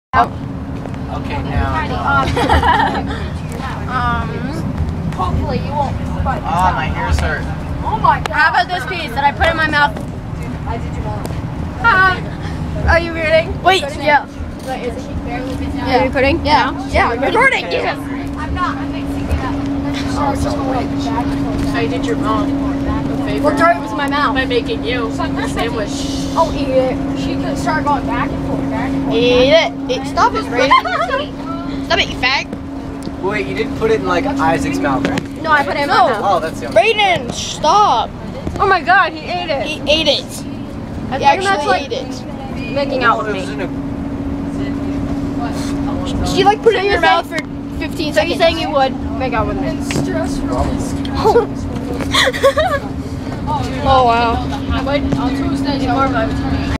Meanwhile, On The Bus
This guy in my school eats an airhead that's been slobbered on by this chick!